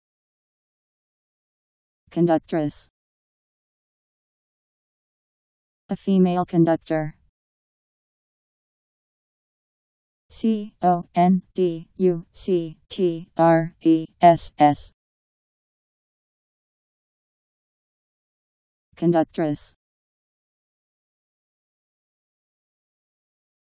Conductress Meaning
Video shows what conductress means. a female conductor.  Conductress Meaning. How to pronounce, definition audio dictionary. How to say conductress. Powered by MaryTTS, Wiktionary